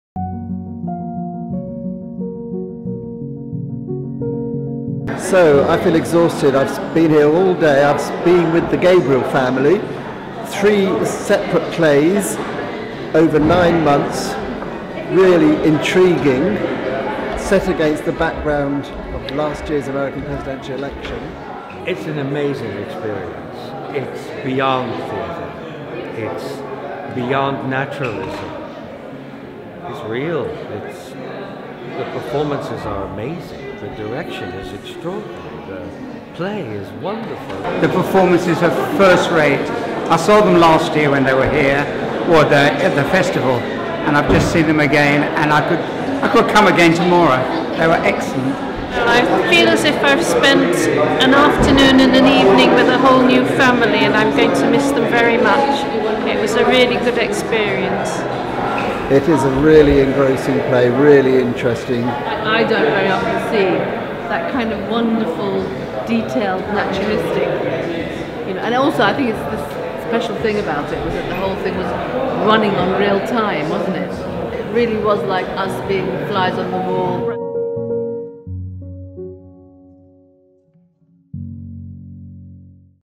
Why audiences love The Gabriels at Brighton Festival
'An extraordinary theatrical event in which the personal and the political combine in a way that suggests a contemporary Chekhov…..[The Gabriels are] deeply moving portraits of the dissolving American dream' ★★★★★ The Guardian

'A theatrical miracle…one of the loveliest and most profound things I’ve ever seen' ★★★★★ What’s on Stage

'Sad, funny, political, deeply intelligent and richly human, this fabulous trilogy of plays set at a family gathering is a huge achievement' ★★★★  The Times